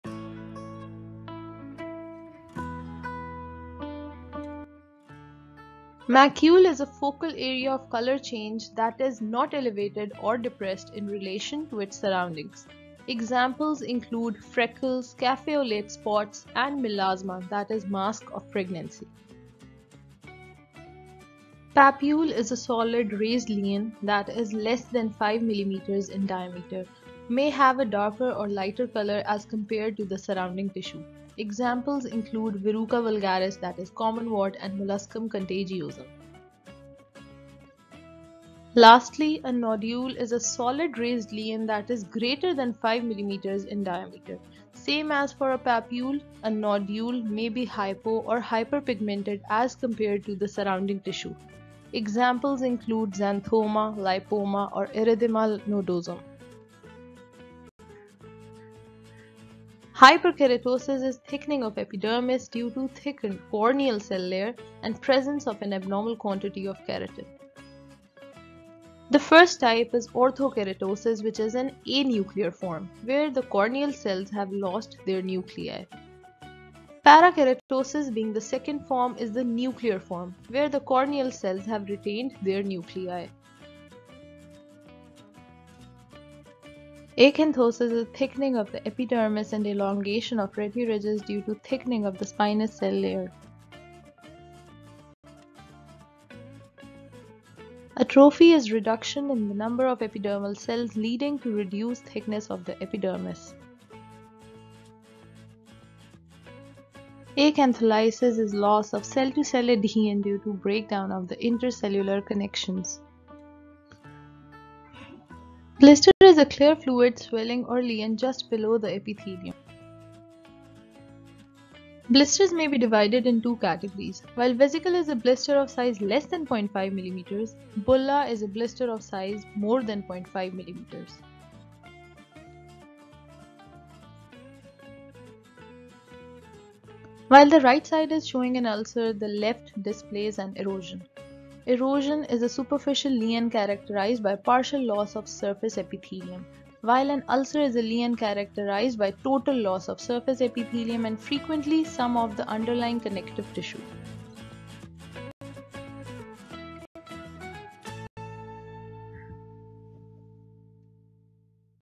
SKIN & MUCOSAL TERMINOLOGIES | ORAL PATHOLOGY BASICS | BASIC VIVA QUESTIONS
The Aim of This Video Is to Explain Common Medical/Dental Terms and to Clarify Any Misconceptions That May Be Present.
Vivas May Start from Basics and Not Knowing a Very Basic Term May Lead to a Bad Impression, While Even Having a Hint May Save You from any Such Situations.

For anyone who might be affected by any disease sharing a link of a non-medical treatment method called as "THE ULTIMATE REMEDY"

 @MAST MAST HEALERS

ARTIST: REED MATHIS
FROM: YOUTUBE LIBRARY